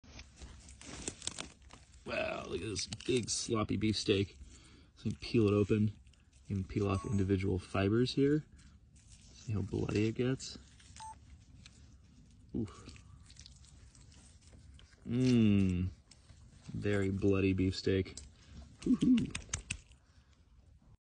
Big Sloppy Beefsteak Mushroom (Fistulina hepatica), looks like raw beef, tastes like lemon!
Bloody week continues 🌧🩸, I found this massive growth of mature BeefsteakFungus Fistulinahepatica growing out of a Chinquapin Oak up in Mendocino. This is a weakly parasitic brown rot fungi that usually favors hardwoods. It’s Polypore bracket fungi that grows from a small brownish/red gooey lump into a spreading liver/tongue looking growth. Often there is a sticky wet tacky layer on top, while bottom side is covered in small parallel tubes that disperse spores (similar in concept to boletes). The internal fibers of the mushroom radiate from the tree outwards. When sliced or viscerally ripped open (as in this video) these fibers take on the appearance of bloody raw beef, dripping with red juice. Despite its gory appearance, this is actually a tasty edible mushroom that has a light lemony flavor. When raw it has a texture somewhere between raw tuna and ripe tomato, when cooked it has mild flavor and is crunchy. Genetic studies have placed this in the same clade at Split-Gills (Schizophyllum).
This was by far the largest and most mature beefsteak I’ve seen. It was easily 3.5 ft across and was quite soggy, but still looked remarkably like a massive slice of organ meat coming out the side of a tree. This one was too old to bother eating, but was still worth giving a squeeze to see how truly bloody it is 🥩🩸🍄